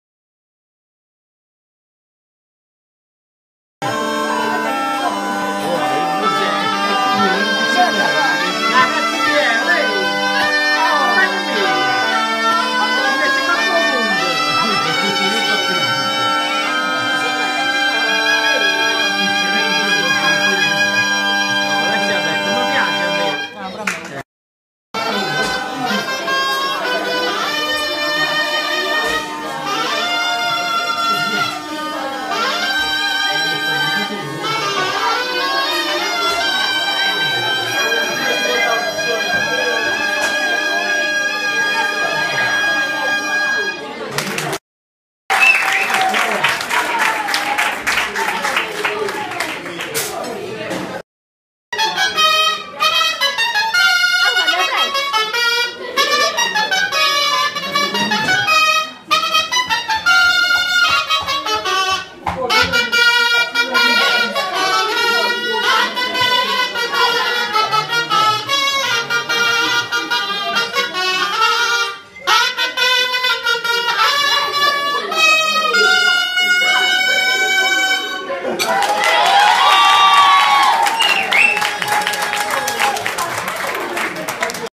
ZAMPOGNA & ZAMPOGNARI UNA LUNGA STORIA DI TRADIZIONI
UNA TRADIZIONE CHE NASCE DAL PROFONDO SUD E ANCORA OGGI VIVE QUAESTO MOMENTO NEL PERIODO NATALIZIO